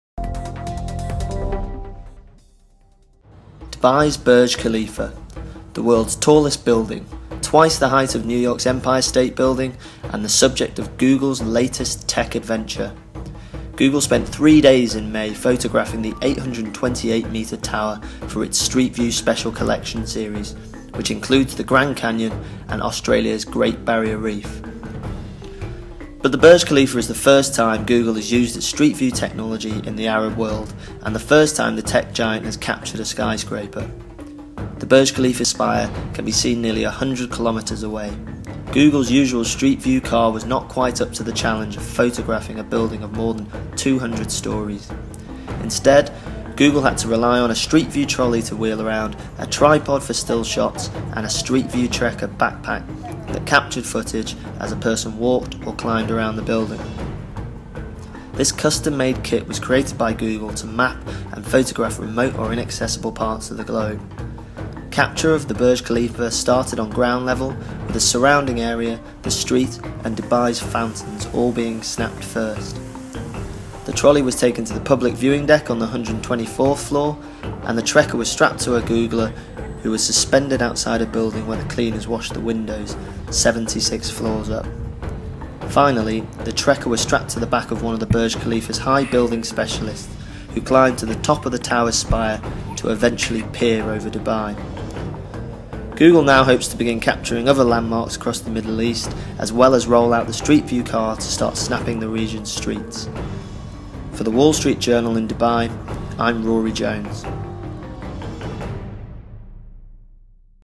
Google Maps sent a team to photograph Burj Khalifa Skyscraper, Burj Khalifa Tower, Burj Dubai Video
Google recently sent a team to photograph the tallest buidling in the world, Dubai's Burj Khalifa Tower, for its Street View Special Collections. WSJ's Rory Jones reports.